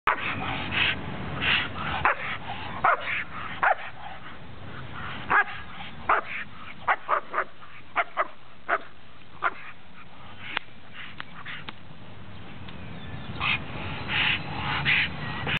EN VENTA / FOR SALE HEMBRA NEGRA SOLIDA LINEA DE TRABAJO
EXCELENTE HAMBRA LINEA DE TRABAJO , PADRES IMPORTADOS DE ALEMANIA, SUPER REPRODUCTORA Y CARACTER A TODA PRUEBA EN PROTECCION.

Working Dogs
Mexico City!!!